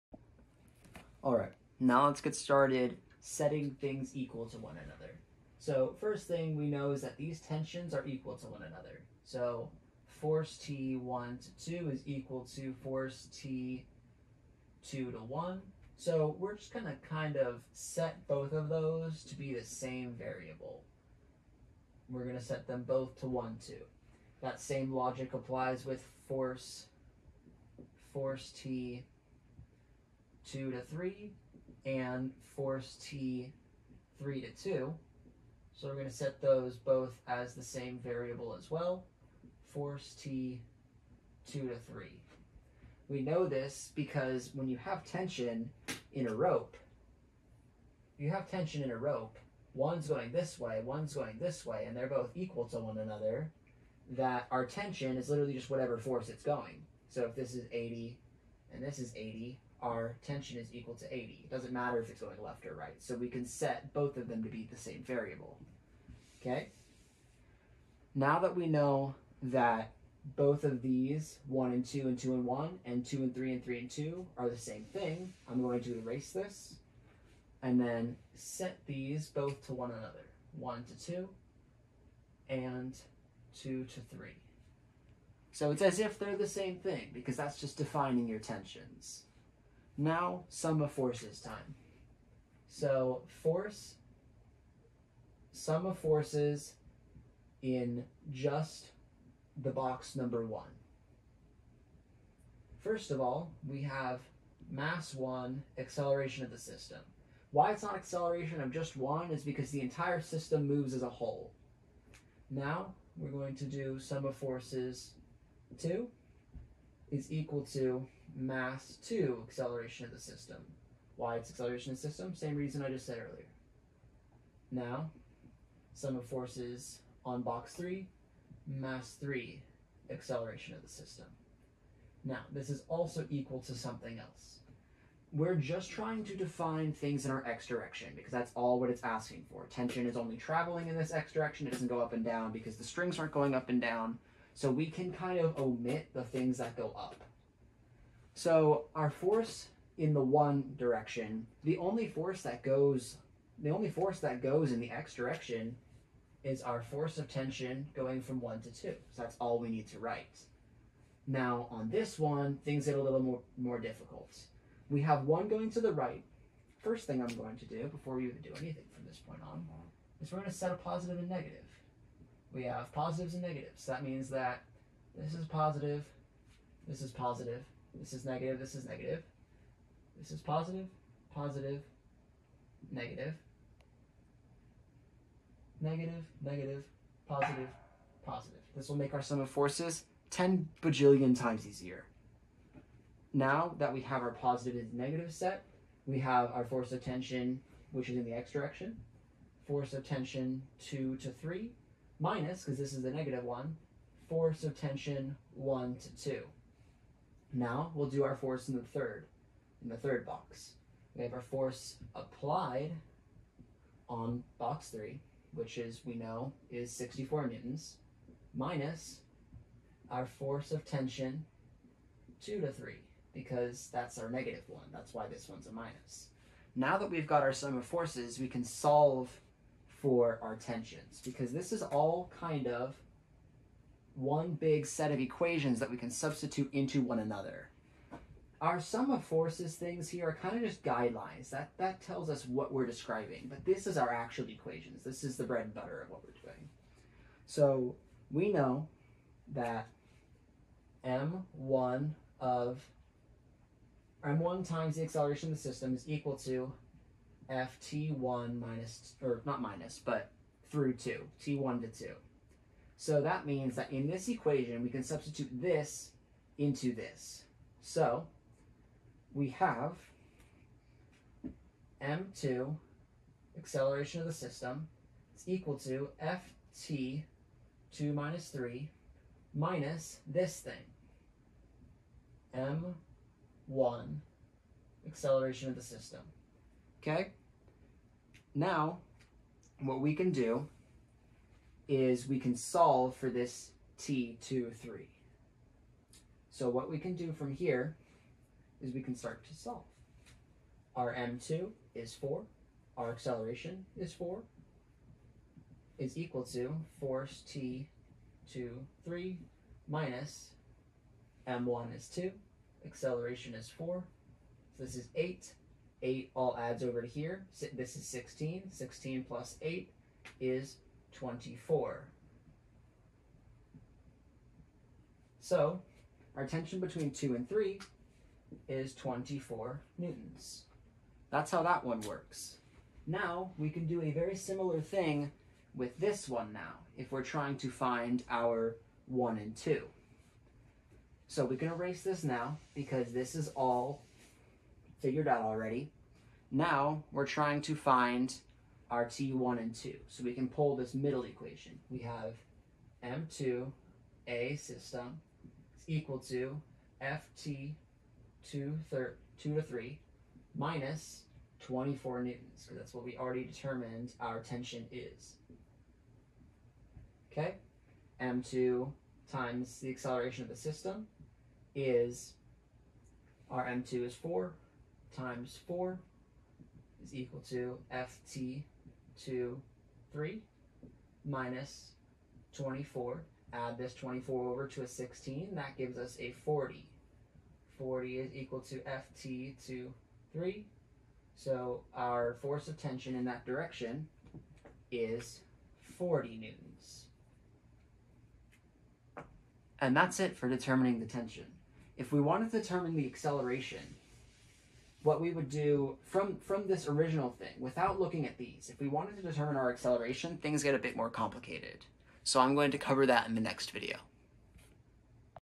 [ΣF, T] Composite Force Systems: Object Method
How to find the sum of forces equations and tension of each individual object, given their masses, the applied force, and the FBDs.